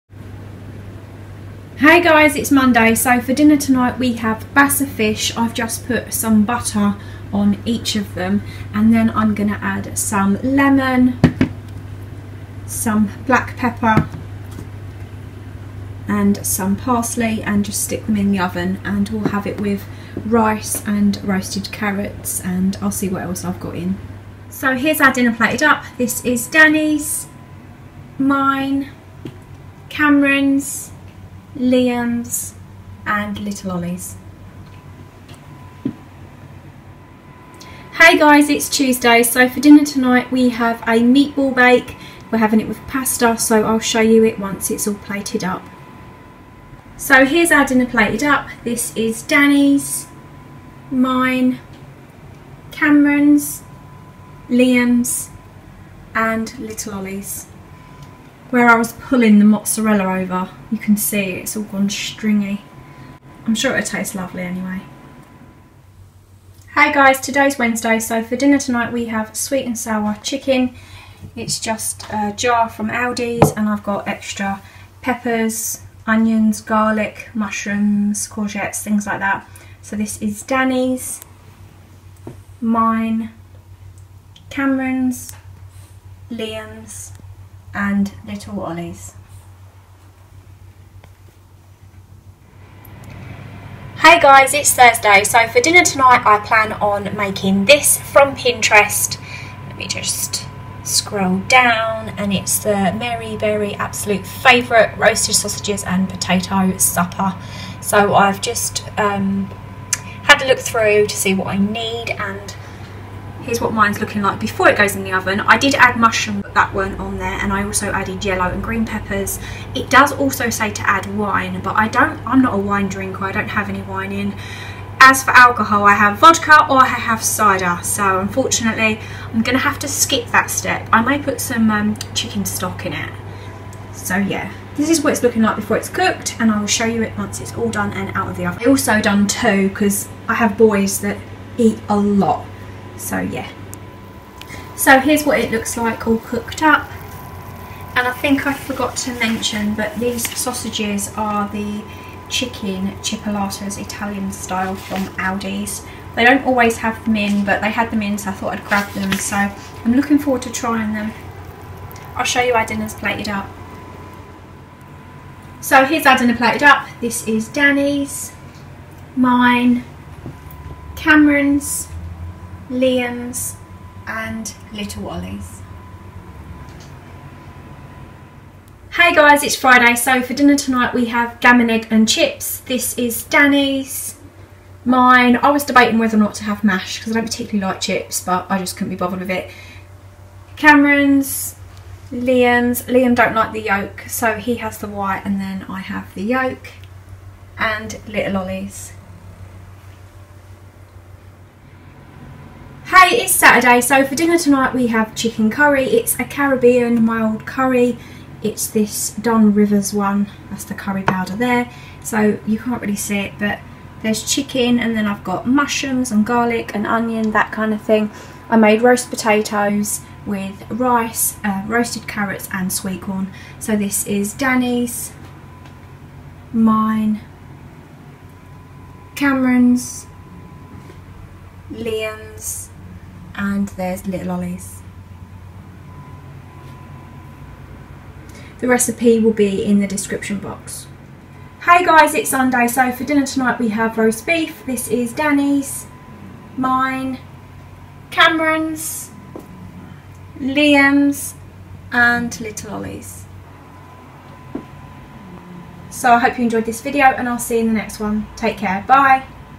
Week of family Meals 15/1-21/1
My family of 5 Meal ideas keeping to a budget

Recipes